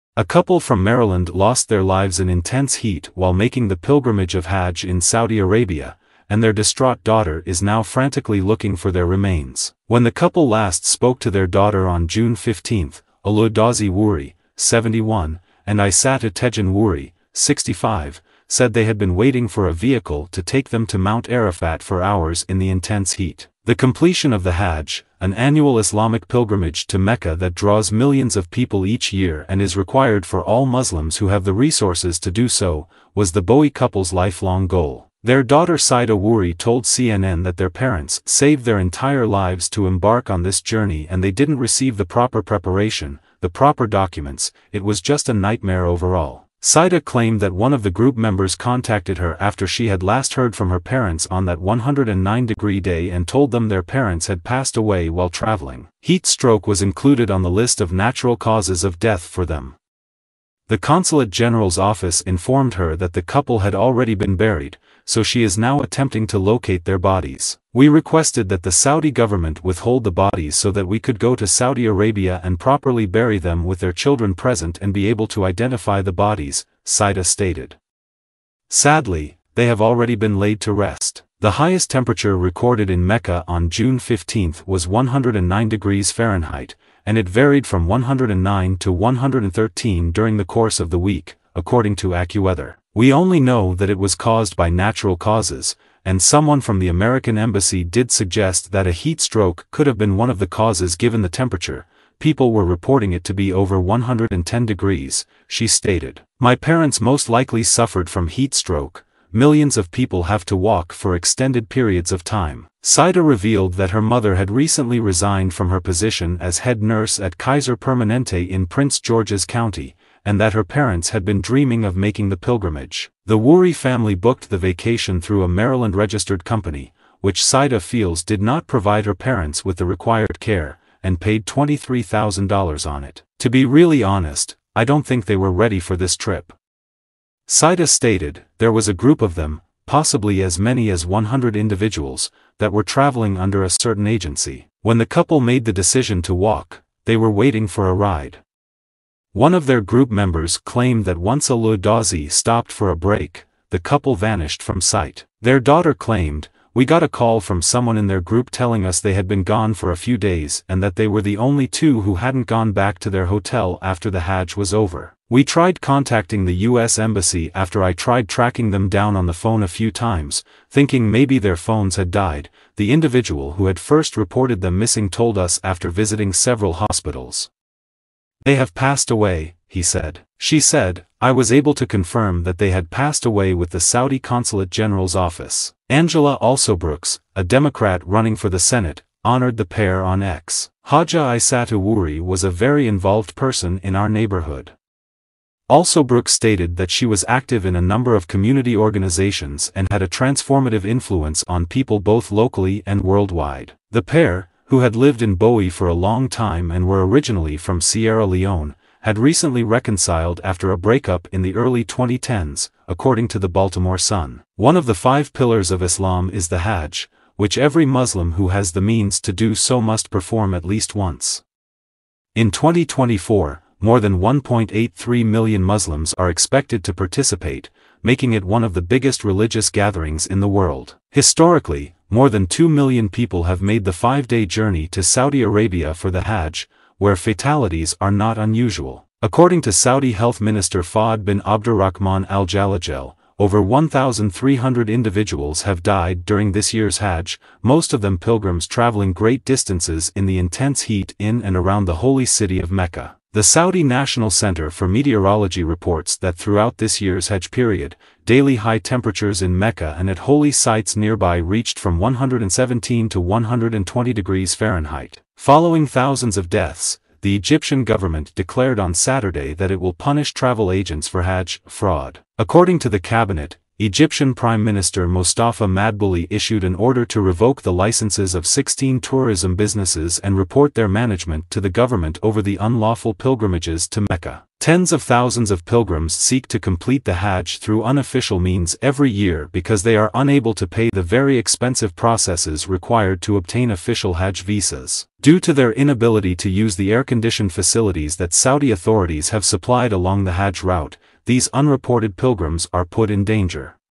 "Tragic Deaths During Hajj: Maryland Couple's Heatstroke Tragedy"
"Discover the heartbreaking story of Alieu Dausy Wurie and Isatu Tejan Wurie, a Maryland couple who tragically passed away during their Hajj pilgrimage due to extreme heat. Their daughter Saida Wurie shares their journey, highlighting the challenges and inadequate preparation they faced. Learn more about this poignant incident and the realities of the annual Islamic pilgrimage."